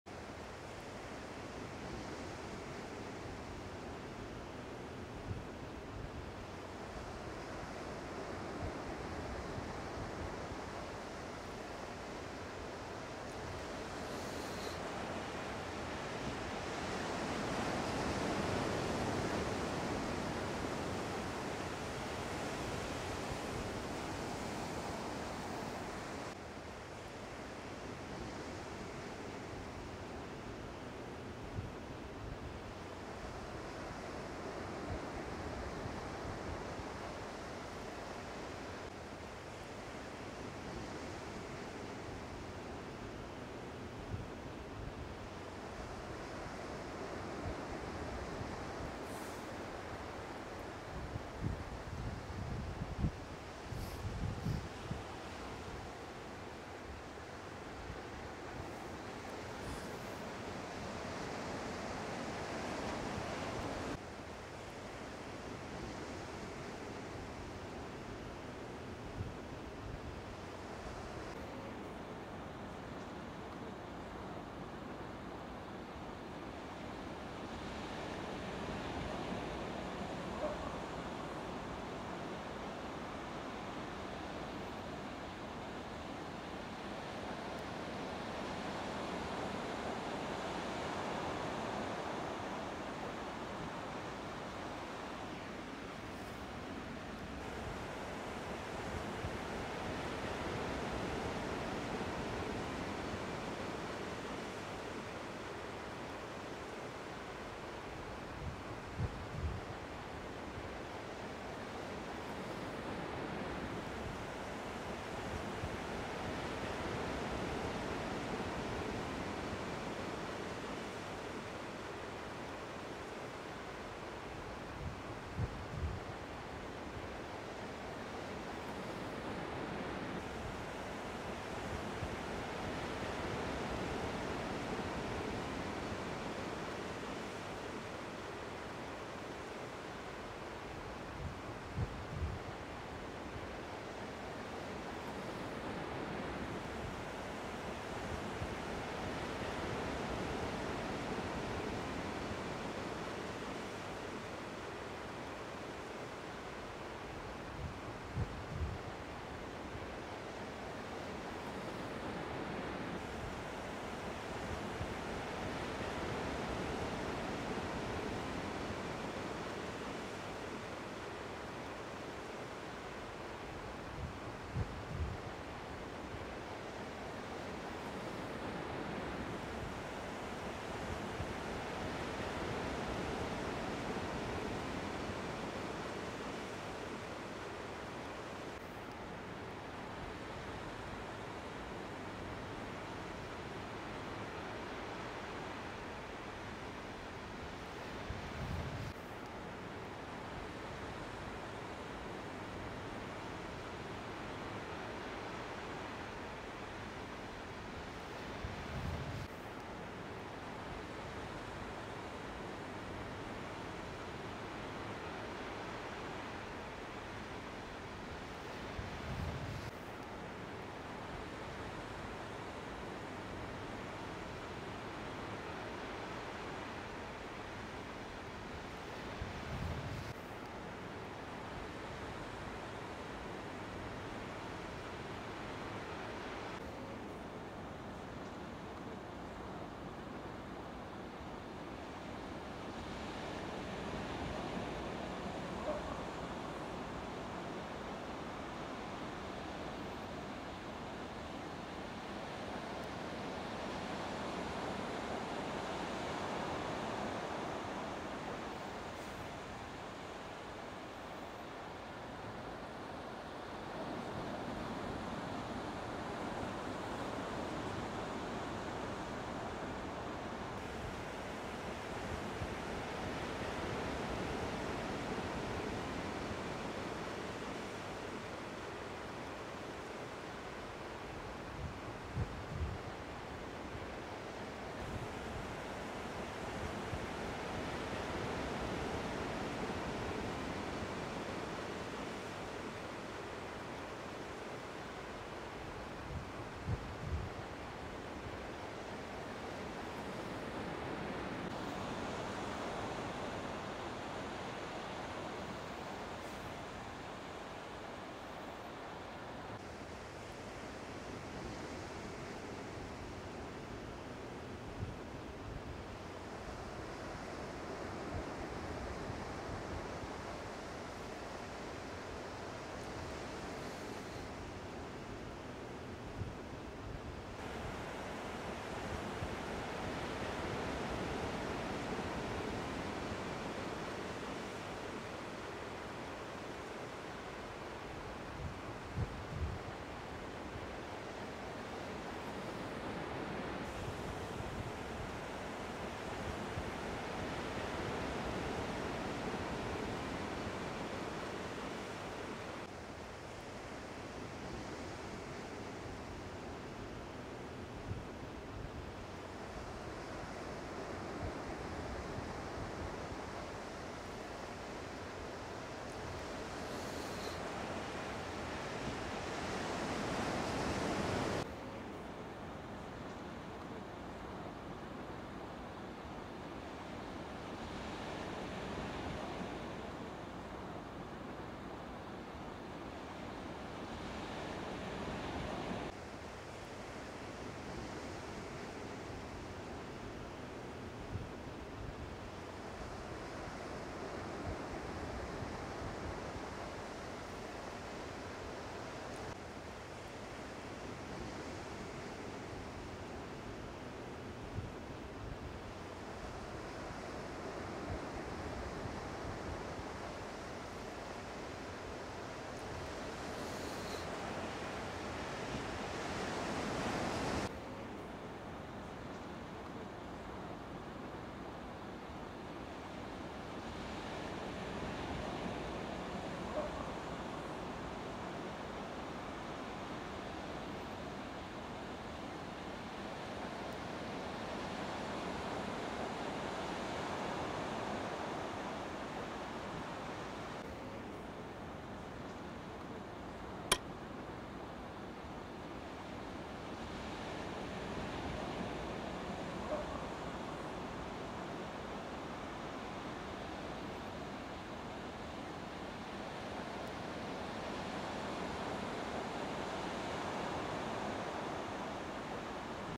Dolphins Playing in the Surf - RAW Drone Footage
It's always great when the dolphins turn up for a surf.

I've been lucky enough to be at the point at the right time with my done to capture them on 3 occasions.

Here's a video of the RAW footage captured.